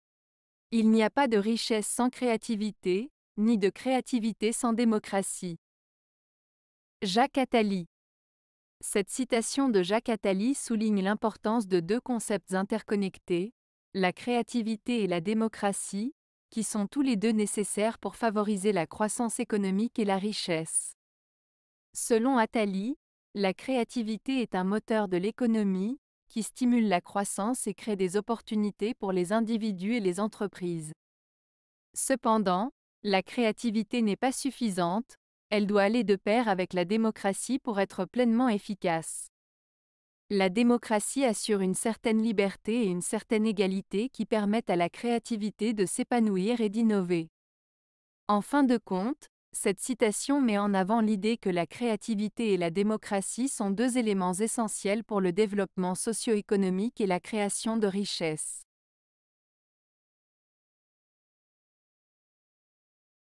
Il n'y a pas de richesse sans créativité, ni de créativité sans démocratie. - Jacques Attali
Cette citation de Jacques Attali souligne l'importance de deux concepts interconnectés : la créativité et la démocratie, qui sont tous les deux nécessaires pour favoriser la croissance économique et la richesse. Selon Attali, la créativité est un moteur de l'économie, qui stimule la croissance et crée des opportunités pour les individus et les entreprises. Cependant, la créativité n'est pas suffisante, elle doit aller de pair avec la démocratie pour être pleinement efficace. La démocratie assure une certaine liberté et une certaine égalité qui permettent à la créativité de s'épanouir et d'innover. En fin de compte, cette citation met en avant l'idée que la créativité et la démocratie sont deux éléments essentiels pour le développement socio-économique et la création de richesse.